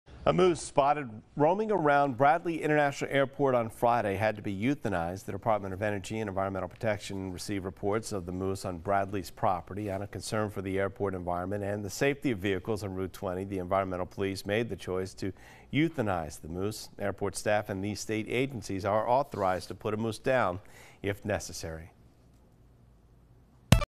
Moose walking around Bradley International Airport euthanized
A moose that was spotted roaming around Bradley International Airport on Friday had to be euthanized.